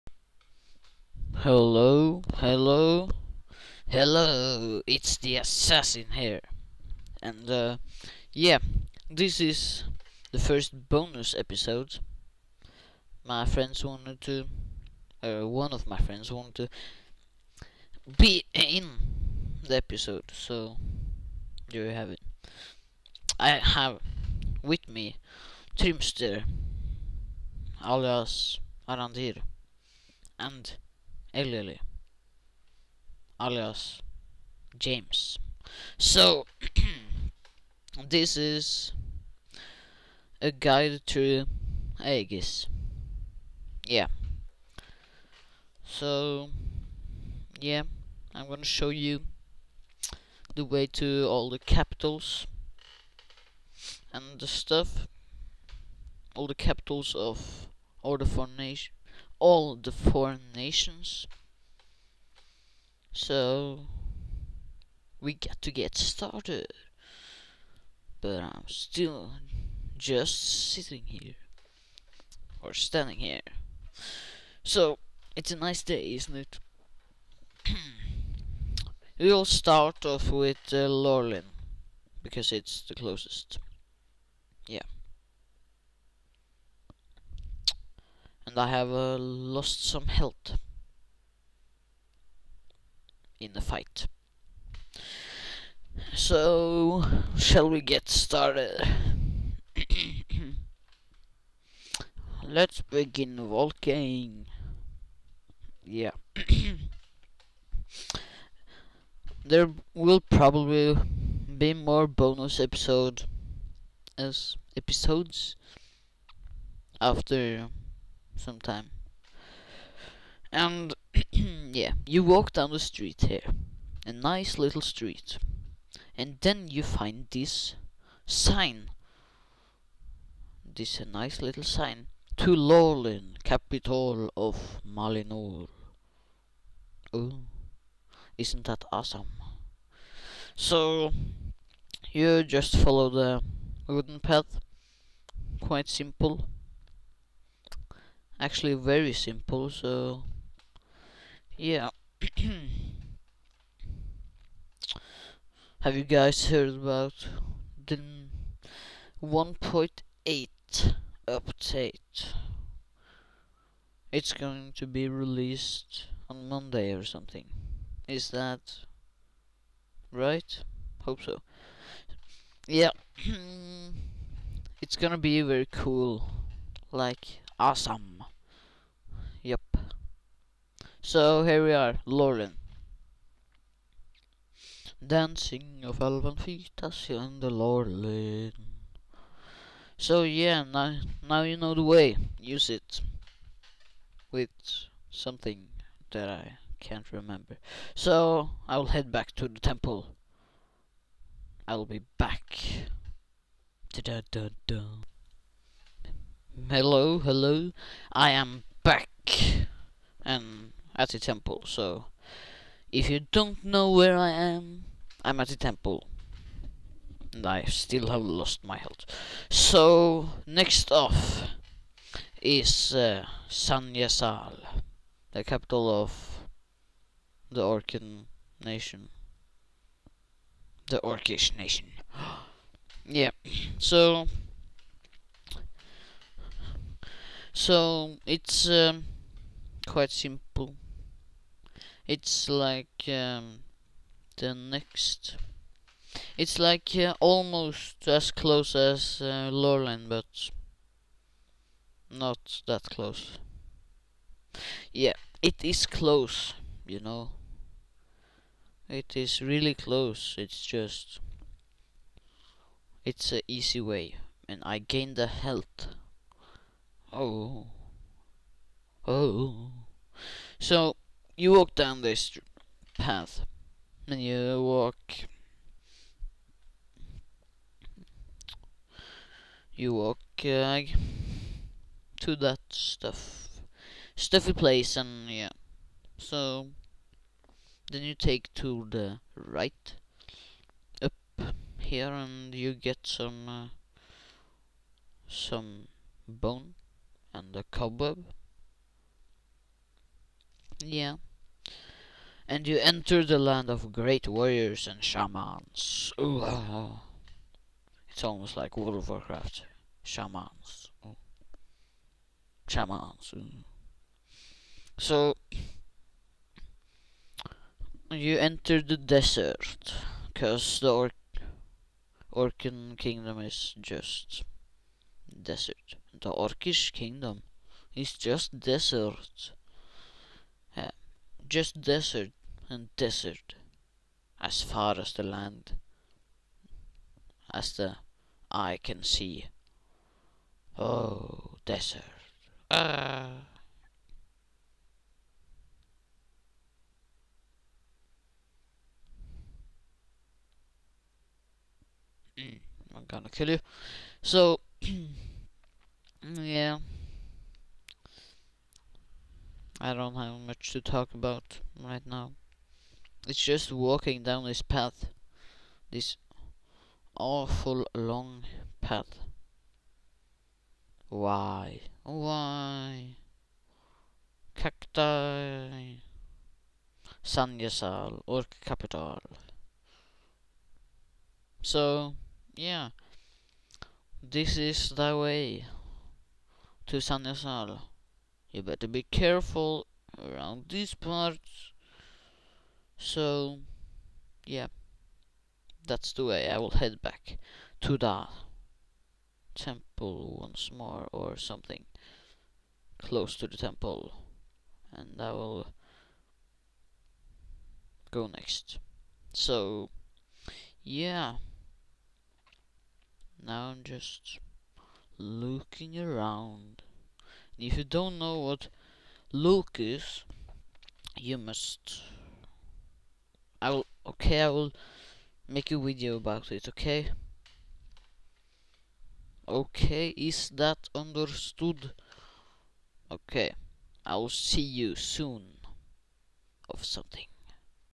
Lord of The Craft Bonus.1 A guide through Aegis (Part 1.)
The Assassin shows himself once more, but this time in a bonus episode!
He shows you the way to all of the capitals of Aegis!